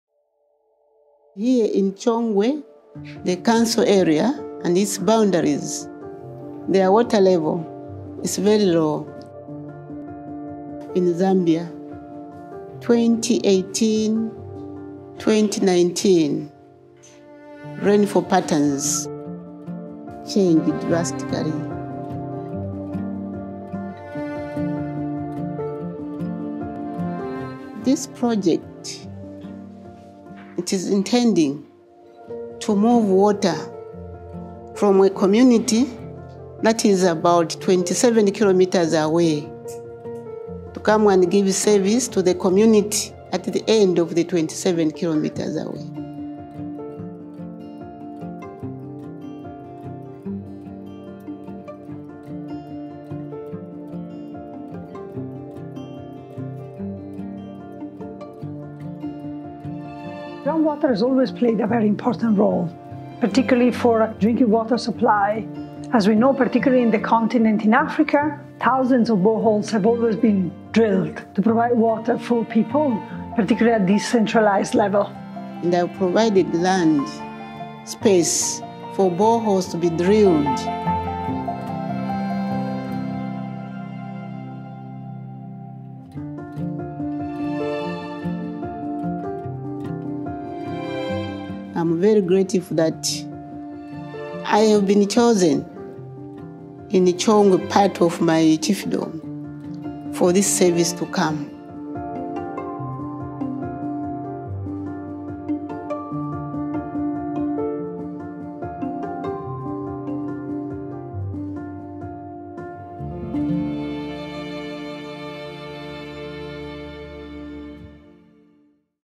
GROUNDWATER IN ACTION : A PILOT PROJECT IN ZAMBIA GIVES HOPE TO THE CHONGWE COMMUNITY
Groundwater is a vital source of water supply to manage the growing demand in Zambia due to extended droughts in the country. However, groundwater, often unmeasured and unmanaged, needs to be sustainably managed to maintain the levels of the water table. With support from the Cooperation in International Waters in Africa (CIWA), and partnership with the Global Environment Facility (GEF), the Southern African Development Community (SADC), through its Groundwater Management Institute has initiated a pilot project in Zambia addressing the water shortage by providing groundwater to the Chongwe community.